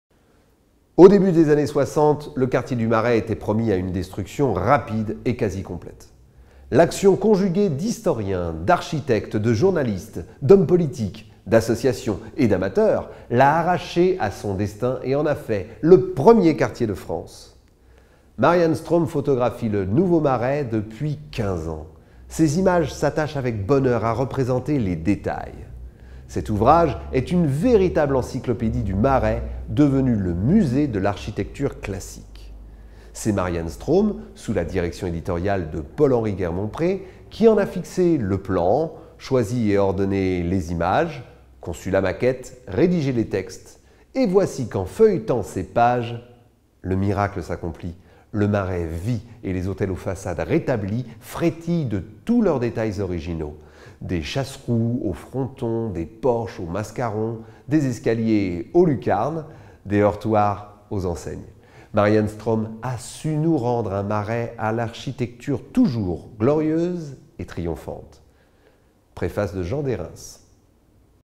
Le Marais
Paul-Henri Guermonprez ÉDITEUR

Au début des années 1960, le quartier du Marais était promis à une destruction rapide et quasi complète ; l’action conjuguée d’historiens, d’architectes, de journalistes, d’hommes politiques, d’associations et d’amateurs l’a arraché à son destin et en a fait le premier quartier de France.

Marianne Ström photographie le nouveau Marais depuis 15 ans. Ses images s’attachent avec bonheur à représenter les détails.

Cet ouvrage est une véritable encyclopédie du Marais, devenu le musée de l’architecture classique. C’est Marianne Ström, sous la direction éditoriale de Paul-Henri Guermonprez, qui en a fixé le plan, choisi et ordonné les images, conçu la maquette, rédigé les textes et voici qu’en feuilletant ses pages le miracle s’accomplit :

le Marais vit et les hôtels aux façades rétablies frétillent de tous leurs détails originaux, des chasse-roues aux frontons, des porches aux mascarons, des escaliers aux lucarnes, des heurtoirs aux enseignes.

Marianne Ström a su nous rendre un Marais à l’architecture toujours glorieuse et triomphante.
Préface de Jean Dérens.

"Aidez Sofian dans son combat contre l'ataxie telangiectasie"